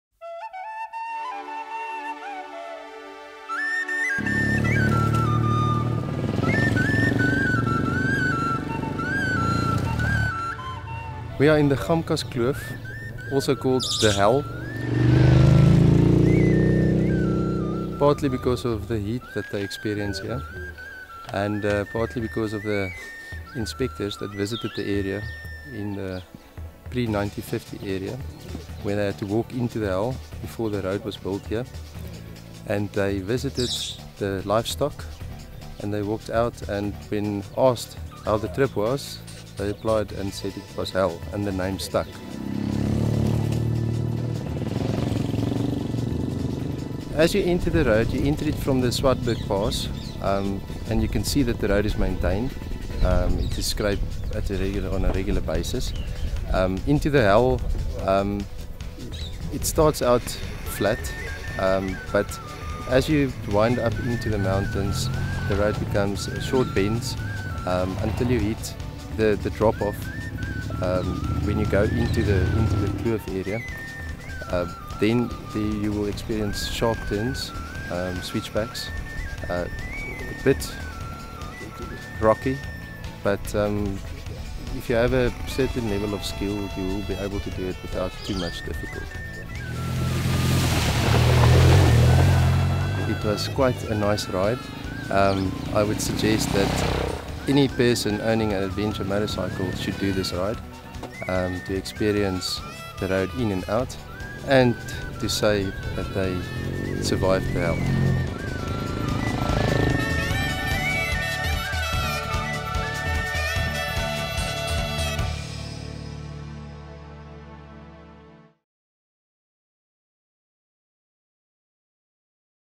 Gamkaskloof/Die Hel with Mathinthinyane Adventures
Mathinthinyane will tailor make a trip for you to Gamkaskloof. The area is also know as Die Hel and is ideal adventure bike territory.